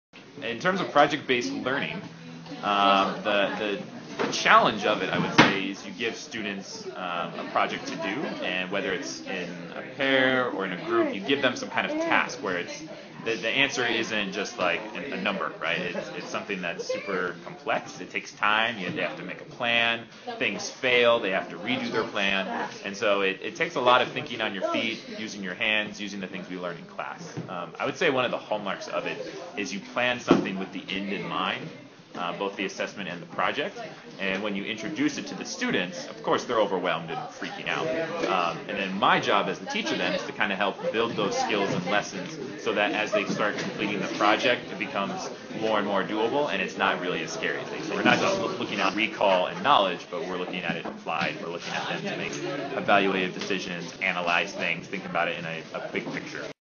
Steve Pratt, High School Teacher, Cleveland High School, Discusses the Future of Science Education
The Future of Science Education: STEM and Workforce Readiness examines the broad set of issues facing STEM education and hone in on project-based learning models. From transforming graduation rates across the board to fostering engagement and interest in STEM subjects, project-based learning is proving its unique value. Watch Steve Pratt and other experts as they discuss these challenges